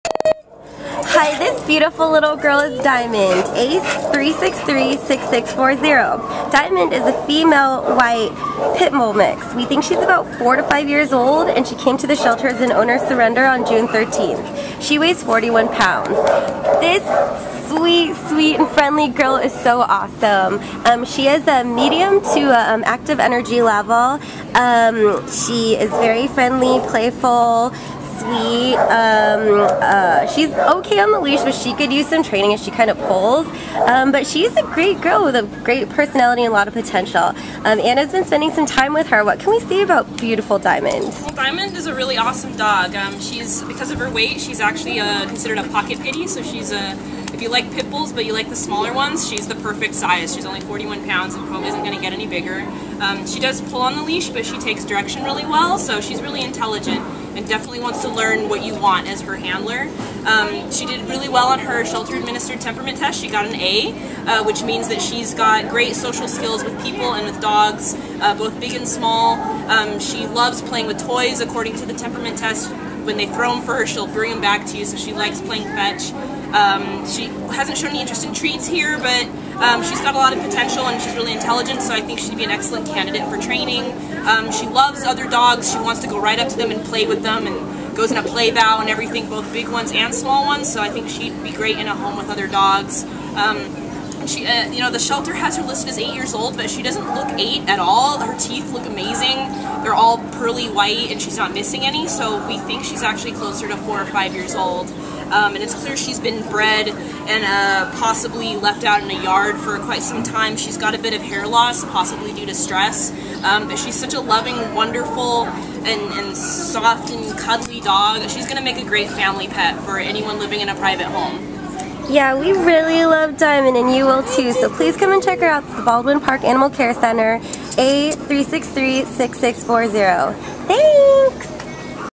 A3636640 Diamond | Pit Bull Mix
A3636640 Diamond is a dazzling 4½-year-old white female Dogo Argentino/Pit Bull mix whose owner left her at the Baldwin Park Animal Care Center on June 13th. Weighing 41 lbs, Diamond is as sparkly and beautiful as her name. She has a winning smile, a friendly attitude, and gets along wonderfully with other dogs of all sizes. She already knows how to sit on command, but would benefit from some additional leash training to polish her manners. It is no surprise that Diamond passed her shelter-administered temperament test with an excellent 24/25 score! Diamond is just an all-around great dog, and will be an excellent friend for anyone seeking a well-socialized, medium-sized dog, including first-time dog owners.